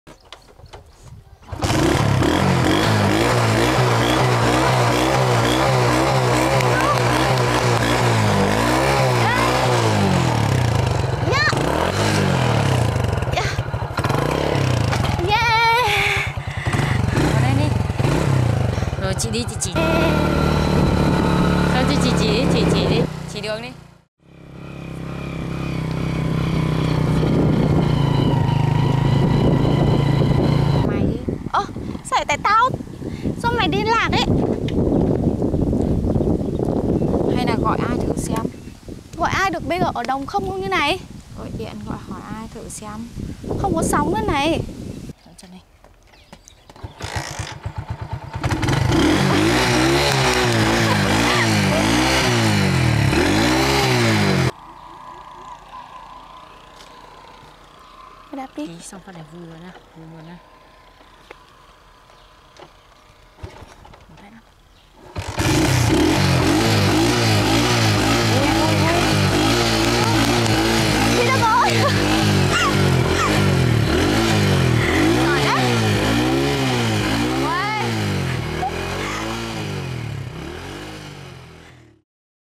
Kickstart motorbike | Hypatia & Tegan Lost their way
Two girls lost their way with a broken old motorbike, blaming each other, upset about the motorbike, causing them to go through a test of the motorbike as well as their trust.